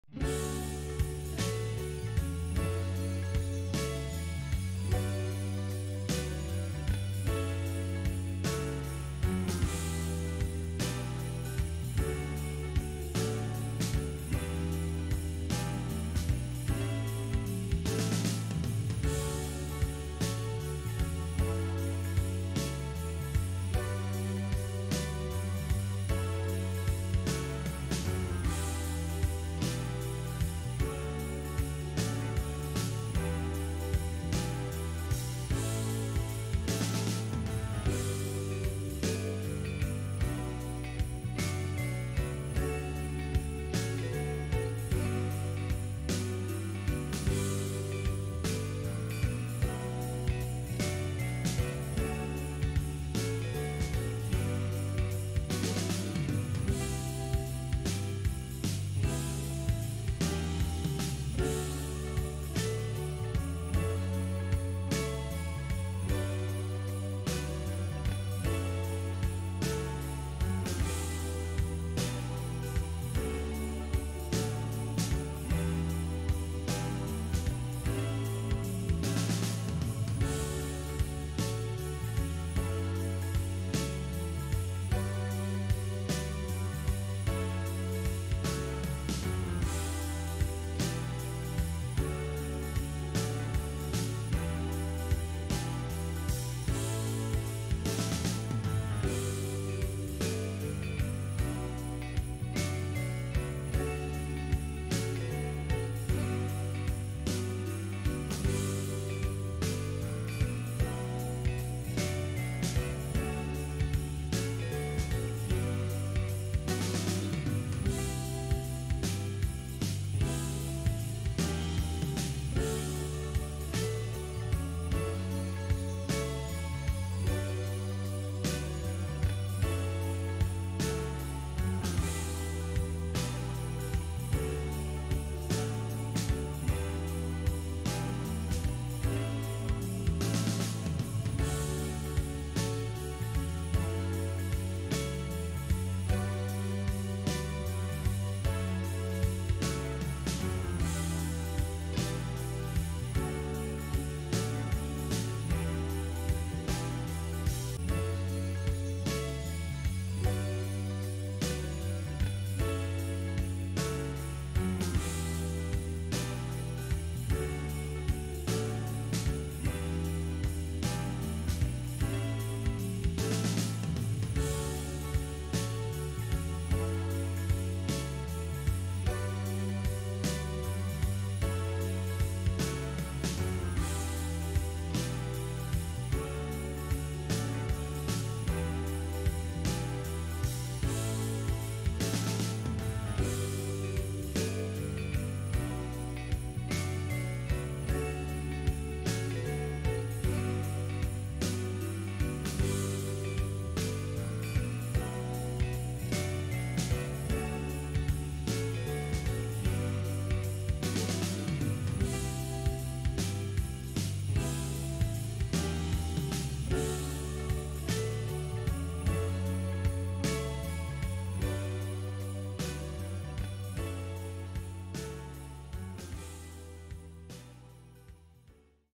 Christmas Tree Hats | Woolly Hat Ornaments | Soft Ornaments | Knit Cap Ornament | Xmas Decoration
With Christmas decorations sometimes simple is best, like in the case of this classic tree hats. This simple Christmas tree decor idea is as easy as it is beautiful. Soft ornaments are a must for any pet owners or parents because they don't shatter when dropped. A hint, put a cotton ball inside the hat to keep its shape.